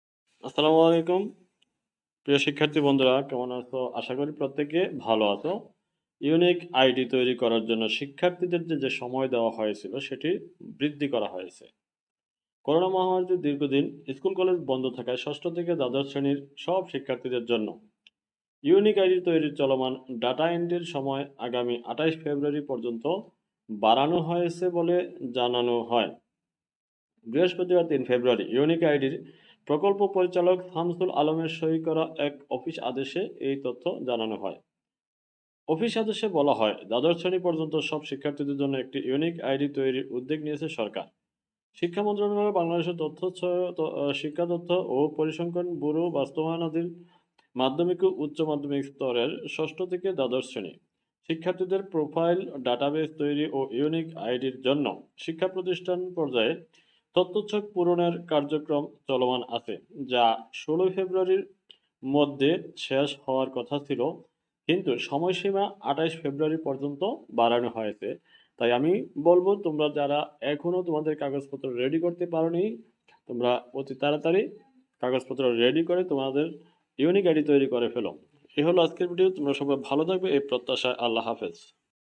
ইউনিক আইডি কার্ড করার সময় বৃদ্ধি করা হয়েছে ll All time teach.
শিক্ষা মন্ত্রণালয়ের নির্দেশনা মোতাবেক সকল শিক্ষার্থীর ইউনিক আইডি কার্ড তৈরি করতে হবে।
প্রত্যেক শিক্ষার্থীর প্রতিটি
ক্ষেত্রে প্রয়োজন।
কলেজে ভর্তি, পরীক্ষার সময়,
ব্যাংক একাউন্ট, জাতীয় পরিচয়পত্র।
১. ইউনিক আইডি কার্ড কেন করবে?
২. ইউনিক আইডি করতে কি কি লাগবে?
পিতা ও মাতার জন্ম নিবন্ধন কার্ড,
জাতীয় পরিচয়পত্র,
শিক্ষার্থীর জন্ম নিবন্ধন কার্ড
এবং দুই কপি ছবি।